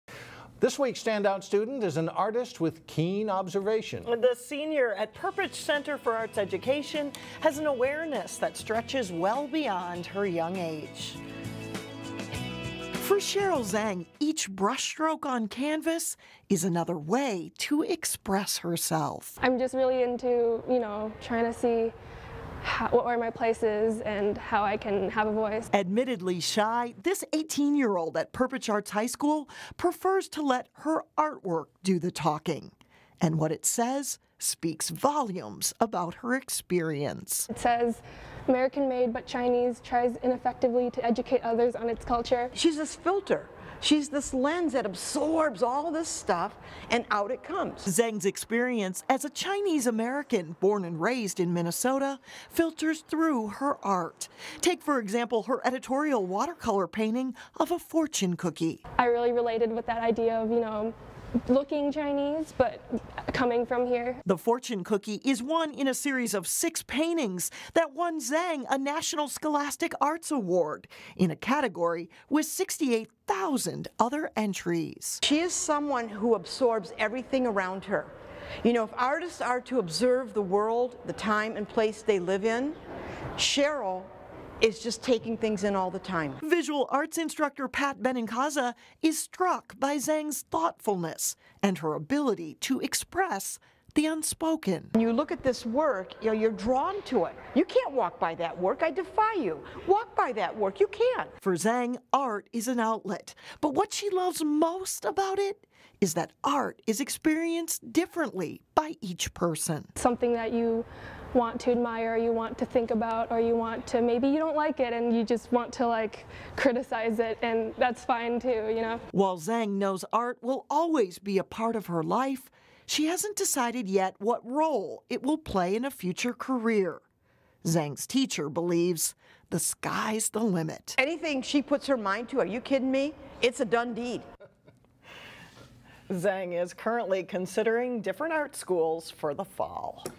Standout Student: Perpich’s Sherrill Zheng
For 18-year-old Sherrill Zheng, each brush stroke on canvas is another way to express herself.
"I'm just really into trying to see where my place is and how I can have a voice," said Zheng, a senior at Perpich Center for Arts Education in Golden Valley.
Admittedly shy, Zheng prefers to let her artwork do the talking, and what it says, speaks volumes about her own experience as a Chinese-American, born and raised in Minnesota.
"She is someone who absorbs everything around her," said Pat Benincasa, visual arts department chair at Perpich. "If artists are to observe the world, the time and place they live in, Sherrill is just taking things in all the time."
Zheng's understanding of her identity as a Chinese-American filters through her art. Take for example her editorial watercolor painting of a fortune cookie, with this inscription: "American made, but Chinese. Tries to ineffectively educate others on its culture."
"I really related with that idea of looking Chinese, but coming from here," said Zheng.
The fortune cookie piece is one in a series of six paintings that won Zheng a national Scholastic Arts Award in a category with 68,000 other entries.
Benincasa is struck by Zheng's thoughtfulness and her ability to express the unspoken.
"She is this filter, she is this lens that absorbs all this stuff and out it comes," said Benincasa.
Art is an outlet for Zheng, but what she loves most about it, is that art is experienced differently by each person.
"It's something that you want to admire, or you want to think about, or maybe you don't like it, and you just want to criticize it, and that's fine too," said Zheng.
While Zheng knows art will always be a part of her life, she hasn't decided yet what role it will play in a future career.
Benincasa believes the sky is the limit for Zheng.
"Anything she puts her mind to, are you kidding me, It's a done deed," said Benincasa.
Zheng is currently considering different art schools for the fall.